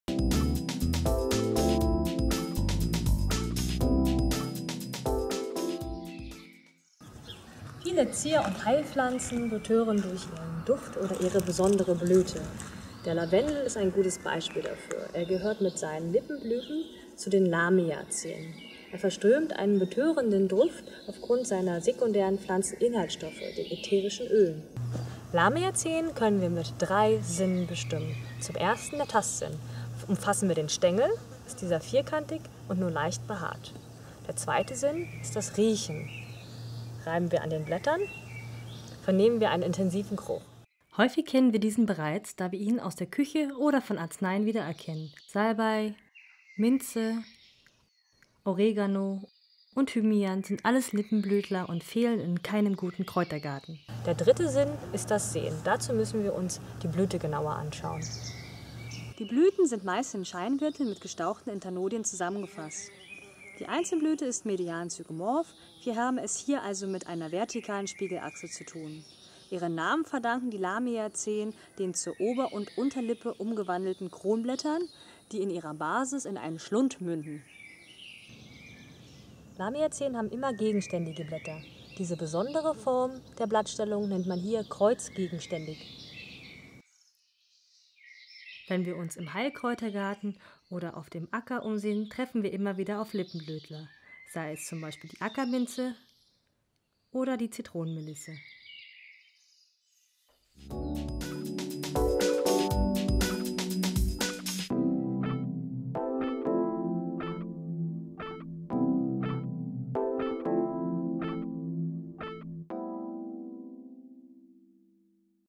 Lamiaceae
Wie erkennen Sie Vertreter der Familie der Lippenblütler? Wir zeigen es Ihnen mit diesem Bestimmungsvideo.